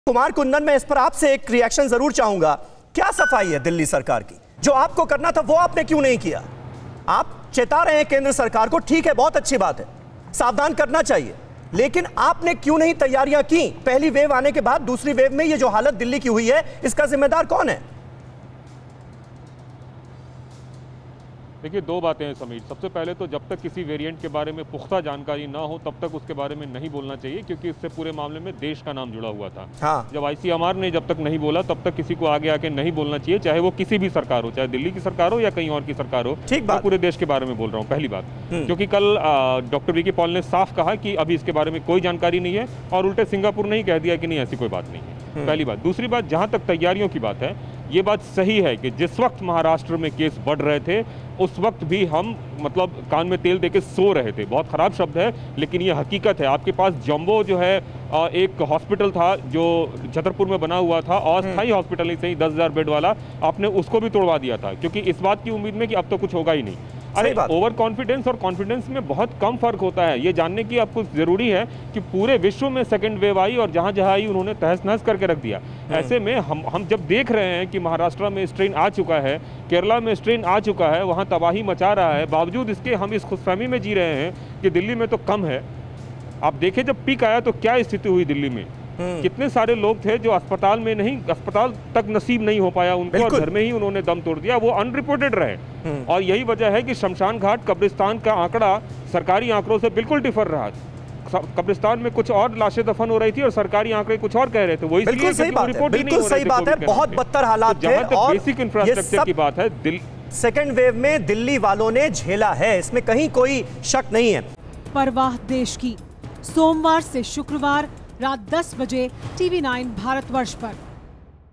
Second Wave में दिल्ली की बुरी हालत के लिए जिम्मेदार कौन ? | Coronavirus
देश में कई राज्यों ने शुरू की तीसरी लहर पर तैयारी. महाराष्ट्र, कर्नाटक में पीडियाट्रिक कोविड सेंटर बनेंगे. यूपी सरकार ने भी थर्ड वेव पर एक्शन प्लान बनाया. यूपी के 45 मेडिकल कॉलेज के 7000 से ज्यादा मेडीकल स्टूडेंट को अस्पतालो में कोविड वार्ड में तैनात किया गया है. यूपी में मेडिकल स्टूडेंट्स को ट्रेनिंग दी गई है.

Credit